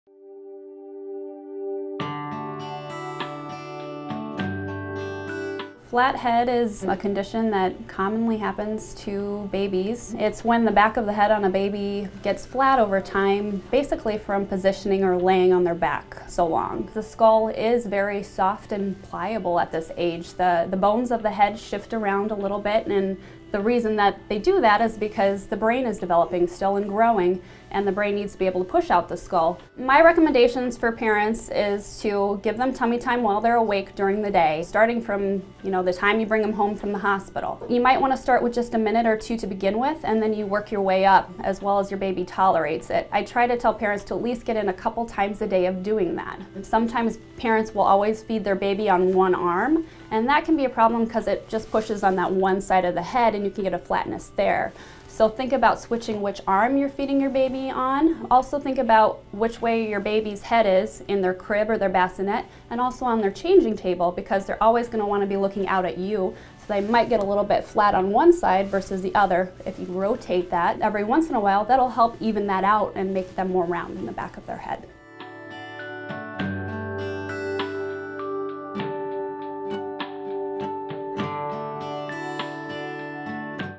Flat Head Syndrome - Boys Town Pediatrics
Flat Head Syndrome is a condition that commonly happens in babies. Dr. Alexis Sawyer, pediatrician with Boys Town Pediatrics, explains what flat head syndrome is and offers recommendations to help prevent flat head syndrome from happening to your baby. For more pediatric tips, visit us